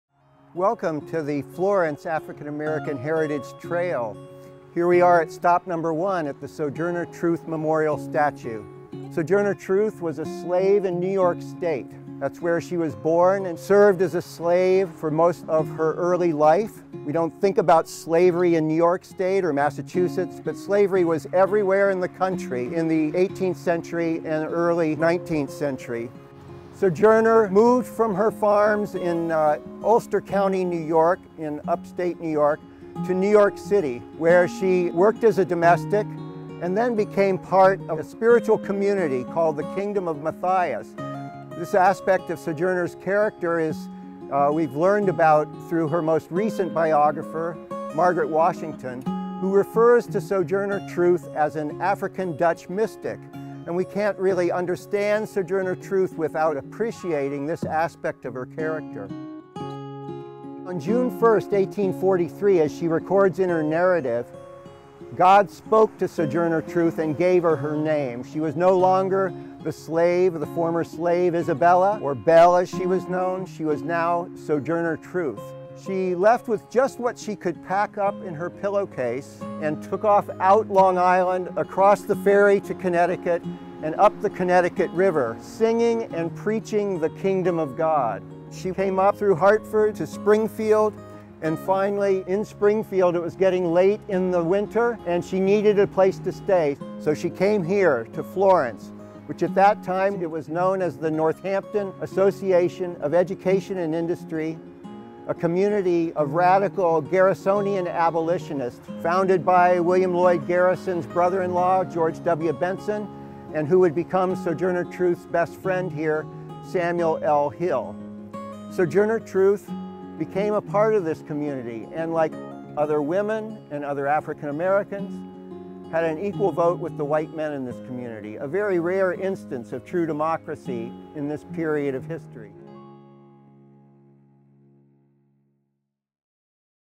Stop #1: Sojourner Truth Memorial Statue
The African American Heritage Trail starts at the Sojourner Truth Memorial Statue, which was dedicated in 2002. Sojourner Truth arrived in Florence in 1843 and joined the utopian society the Northampton Association of Education and Industry.